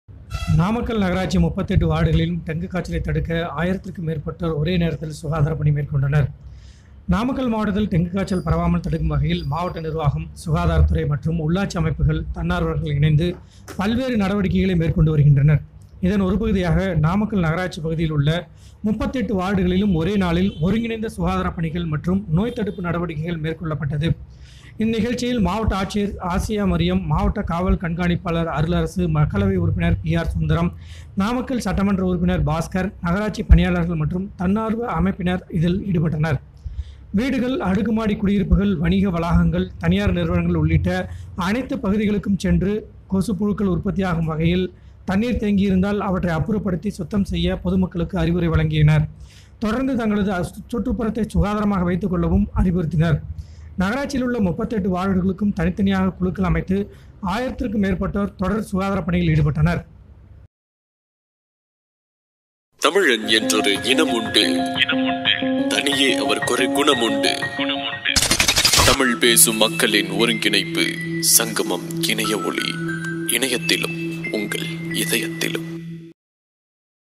நாமக்கல் நகராட்சியில், ஆயிரம் பேர் ஒருங்கிணைந்து சுகாதாரபணி
நாமக்கல் நகராட்சியில் டெங்கு காய்ச்சலை தடுக்கும் வகையில் ஆயிரம் பேர் ஒருங்கிணைந்து சுகாதாரப் பணிகள் மேற்கொண்டனர்.